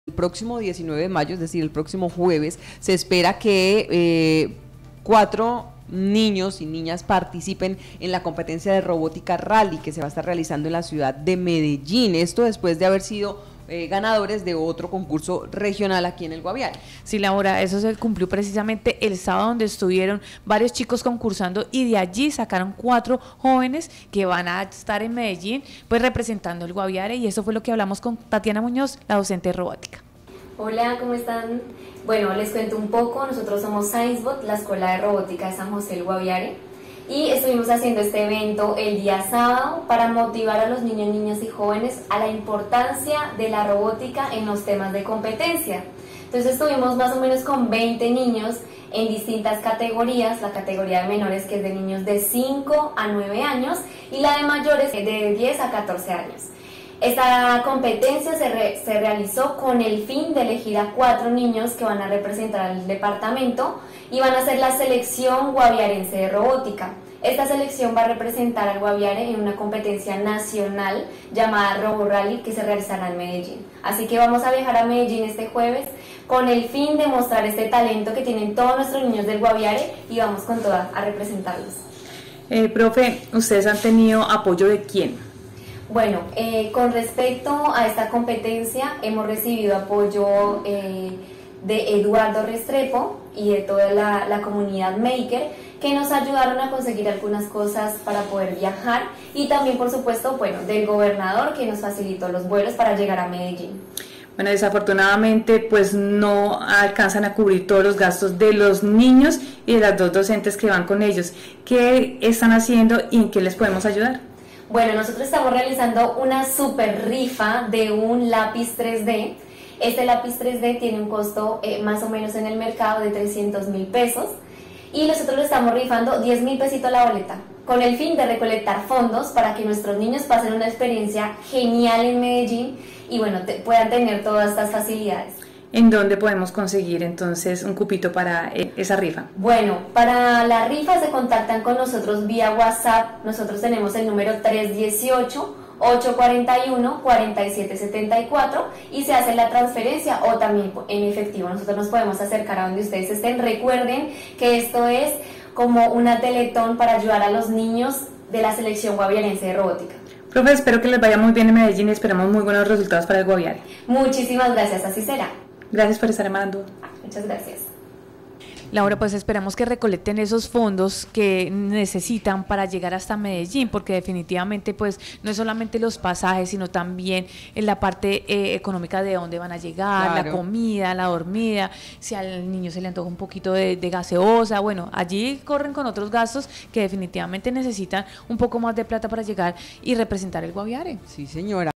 4 jóvenes guaviarenses van para Medellín a competir en Robótica Rally
MaranduaNoticias • 4 jóvenes guaviarenses que van para Medellín a competir en Robótica Rally, este 19 de mayo, aún no cuentan con los recursos para poder cubrir sus gastos del viaje. Desde la escuela de robótica Science Bot se están organizando rifas para poder apoyar a este grupo de jóvenes que irán con sus dos docentes para representar al Guaviare.